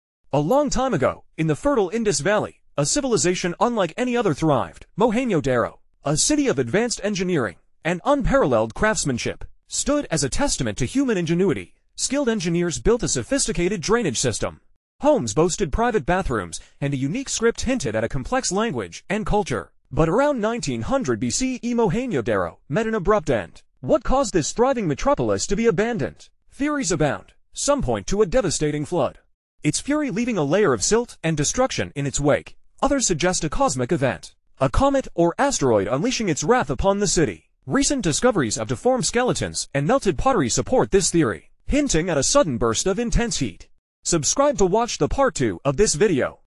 Extinct civilization of India
Mohenjo-daro is a group of mounds and ruins located on the right bank of the Indus River in northern Sindh province, southern Pakistan. It is believed to signify "the mound of the dead" and was first recognized in 1922. The mounds contain the remains of the ancient Indus civilization's largest city, which was considered a capital of an extensive state. Mohenjo-daro was designated a UNESCO World Heritage site in 1980. The city was laid out into a dozen blocks or islands, each about 1,260 feet (384 meters) from north to south and 750 feet (228 meters) from east to west. The citadel carried the religious and ceremonial headquarters. The city was protected by artificial barriers and had a large residential structure, granary, and halls of assembly. The city suffered multiple floods, possibly due to the encroaching Indus and tectonic uplifts between Mohenjo-daro and Harappa.